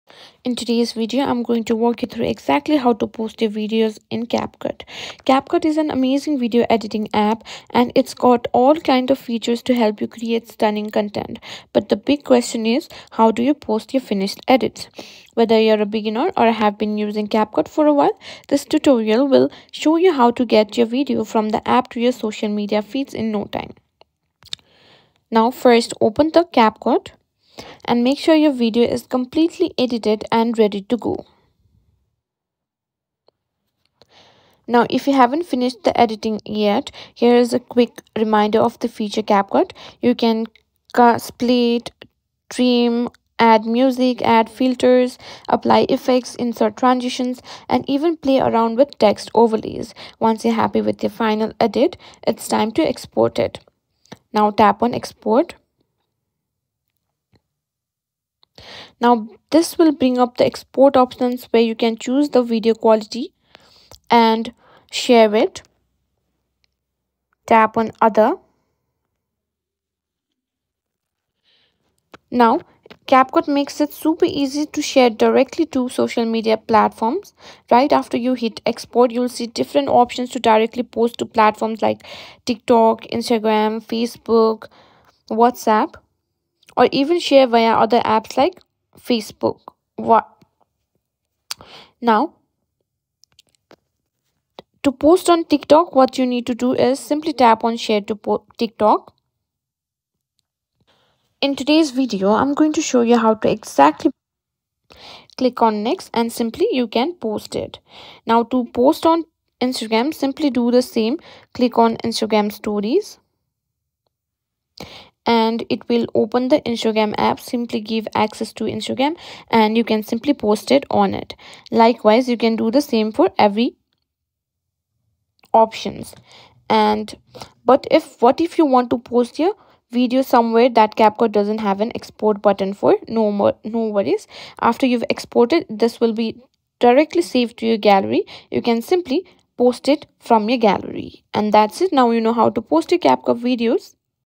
How To Post On CapCut
How to Post on CapCut! Want to learn how to post your creations on CapCut? This video will guide you through the steps on how to post your video projects on CapCut quickly and easily. Whether you're asking, "How do I post my video on CapCut?" or need help with the sharing process, this tutorial has you covered. Learn how to export and share your videos directly from CapCut and make your content available to the world. Watch now to see how you can post your videos on CapCut today!

Timestamps:
00:00 How to Post on CapCut
00:10 How Do I Post My Video on CapCut?